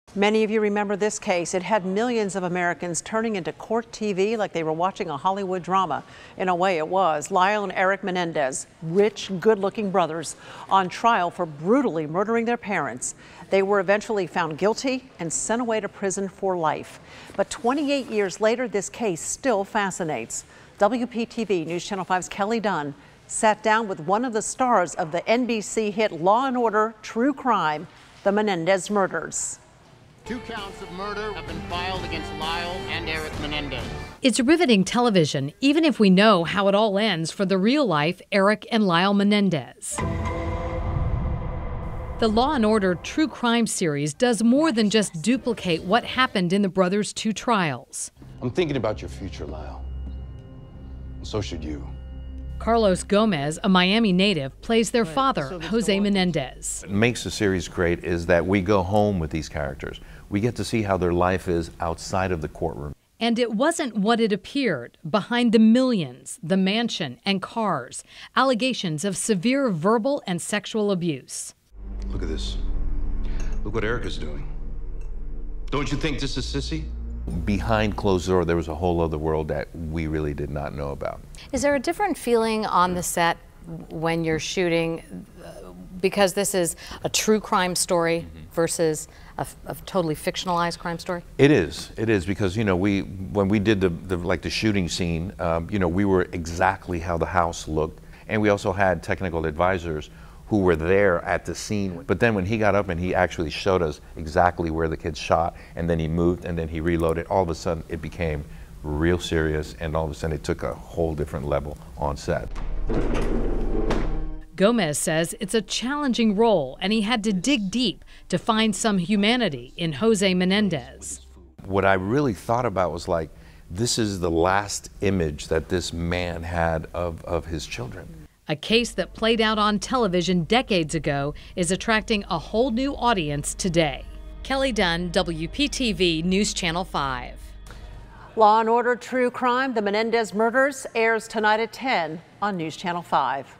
Law and Order True Crime: The Menendez Brothers
WPTV's Kelly Dunn interviews actor Carlos Gomez who plays Jose Menendez in "Law &amp; Order True Crime" story of the Menendez murder case that occurred in 1989.